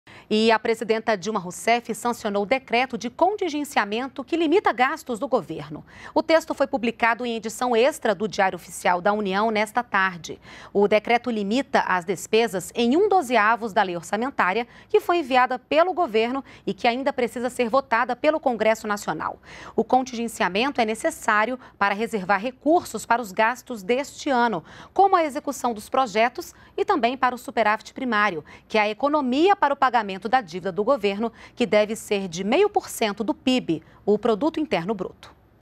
Presidenta Dilma sanciona decreto que limita gastos do governo
NBR NOTÍCIAS - 18.01.16: Nesta segunda-feira, a presidenta Dilma Rousseff sancionou decreto de contingenciamento para limitar gastos do governo. Documento foi publicado no Diário Oficial da União. Medida é necessária para reservar recursos para os gastos de 2016.